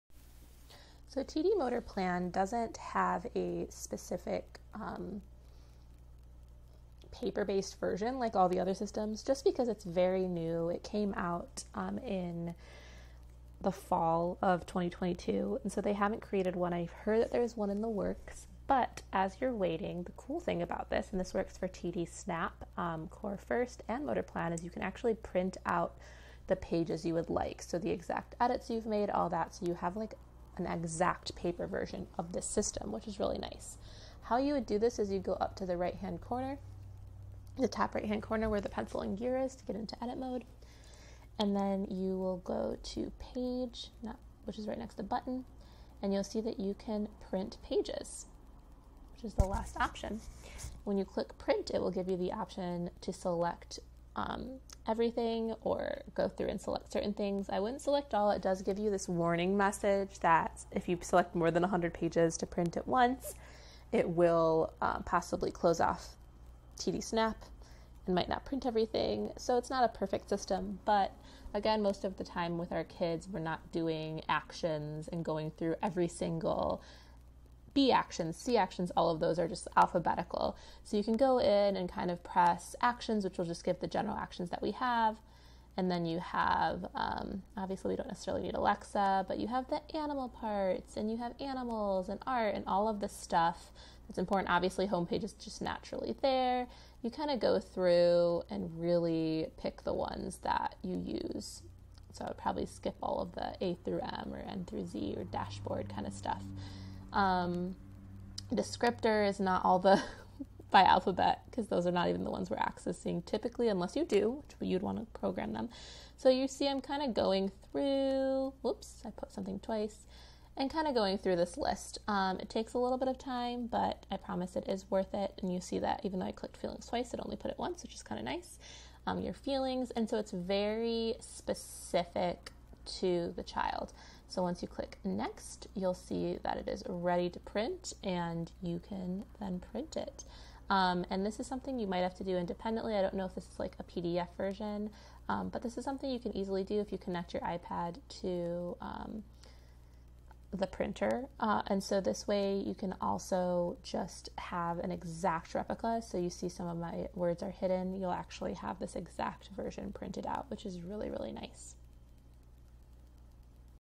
TD Snap Motor Plan: Printing a low tech back up
How do I print an exact copy of my high tech TD Snap Motor Plan pageset? Always have a low tech backup ready and availible for the AAC user to access when the high tech one is not availible!!